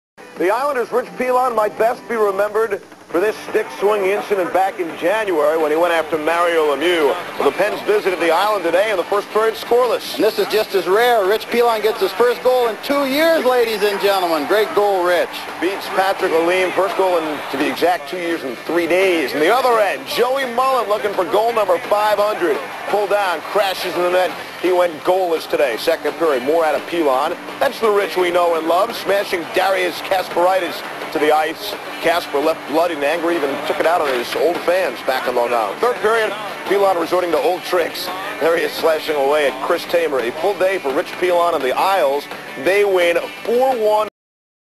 February 23 1997 Penguins at Islanders highlights
1997 NHL ESPN

Kasparaitis Returns To the Coliseum